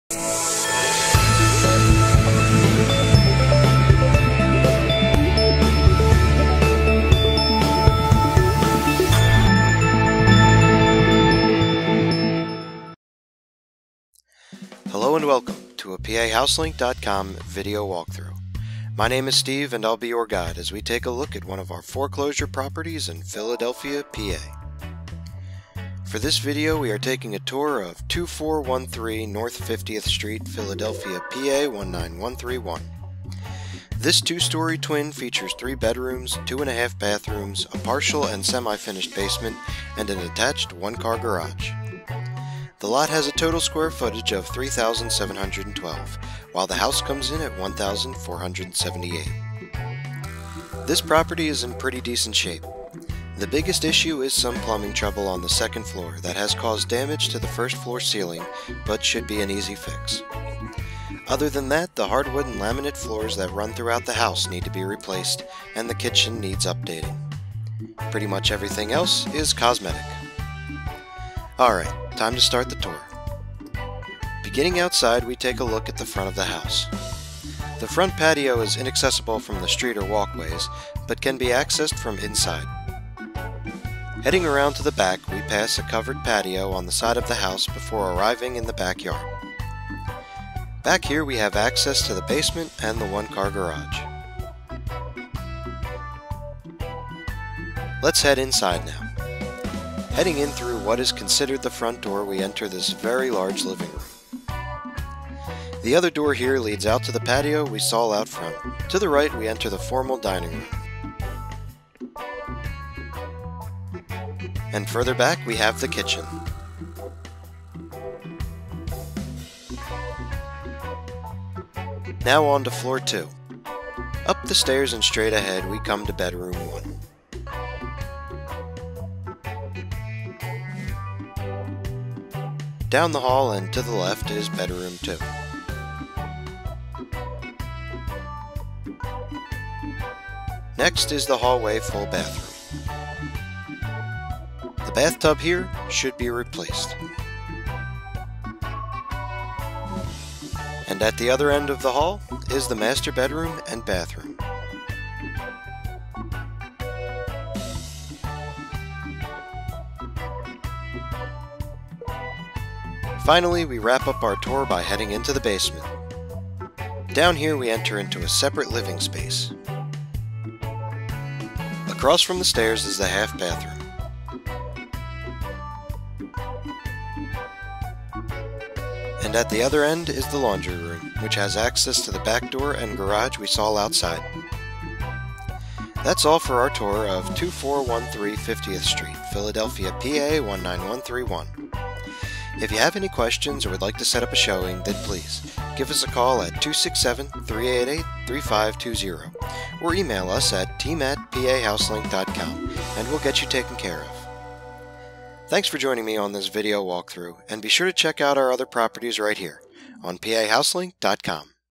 2413 50th St Philadelphia PA 19131 | Foreclosure Properties Philadelphia PA 19131 mp4
2413 N 50th St, Philadelphia, PA 19131 | Foreclosure Properties in Philadelphia, PA
3 Bedroom – 2½ Bathroom 2-Story Twin

Hello and welcome to a PAHouseLink.com video walkthrough. My name is Steve and I'll be your guide as we take a look at one of our foreclosure properties in Philadelphia, PA.

For this video we are taking a tour of 2413 N 50th St, Philadelphia, PA 19131. This 2-story twin features 3 bedrooms, 2½ bathrooms, a partial and semi-finished basement, and an attached 1-car garage. The lot has a total square footage of 3,712, while the house comes in at 1,478.

This property is in pretty decent shape. The biggest issue is some plumbing trouble on the second floor that has caused damage to the first floor ceiling, but should be an easy fix. Other than that, the hardwood and laminate floors that run throughout the house need to be replaced and the kitchen needs updating. Pretty much everything else is cosmetic.

Alright, time to start the tour...

Beginning outside we take a look at the front of the house. The front patio is inaccessible from the street or walk ways but can be accessed from inside. Heading around to the back we pass a covered patio on the side of the house before arriving in the backyard. Back here we have access to the basement and the 1-car garage.

Let's head inside now. Heading in through what is considered the front door, we enter this very large living room. The other door here leads out to the patio we saw out front. To the right we enter the formal dining room. And further back we have the kitchen.

Now on to floor 2. Up the stairs and straight ahead we come to bedroom 1. Down the hall and to the left is bedroom 2. Next is the hallway full bathroom. The bathtub here should be replaced. And at the other end of the hall is the master bedroom and bathroom.

Finally we wrap up our tour by heading into the basement. Down here we enter into a separate living space. Across from the stairs is the half bathroom. And at the other end is the laundry room, which has access to the backdoor and garage we saw outside.

That's all for our tour of 2413 50th St, Philadelphia, PA 19131.

If you have any questions or would like to set up a showing then please give us a call at , or email us at and we'll get you taken care of.